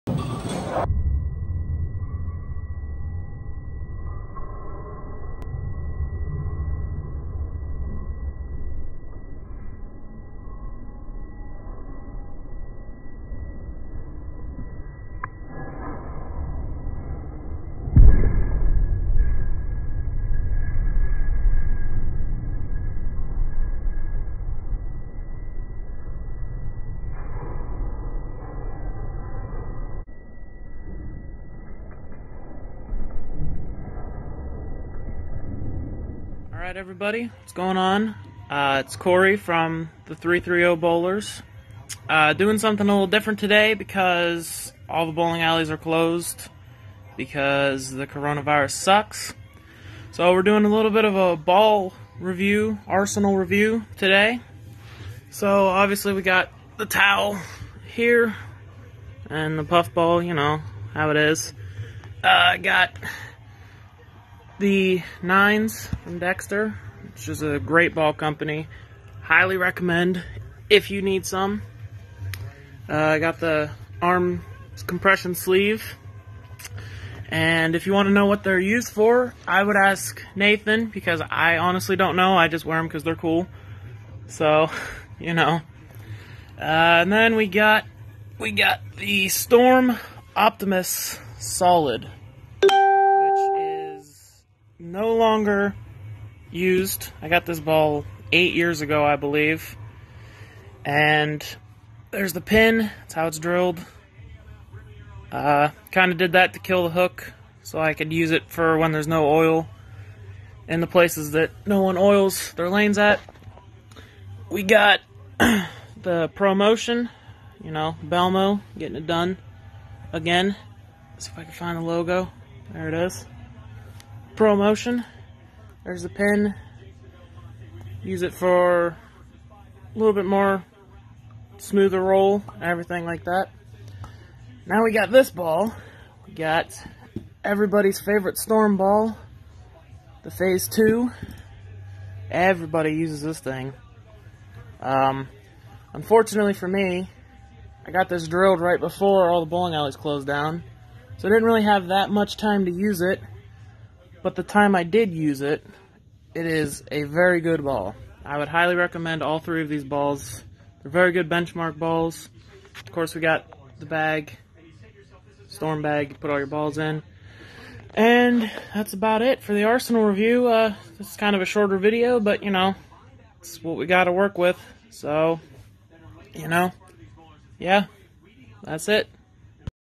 330 BOWLERS ARSENALS | Corey’s Arsenal
Today Corey is showing off his bowling arsenal. This arsenal consists of a storm optimus solid, storm pro motion, and a storm phaze 2.If you life Corey’s arsenal leave a like down below and comment which is your favorite ball. Due to coronavirus COVID-19 we can not get actual shots because the bowling alleys have been shut down. Eventually we will review bowling balls with shots of the ball. Other 330 Bowler arsenals will be shown off over the next couple weeks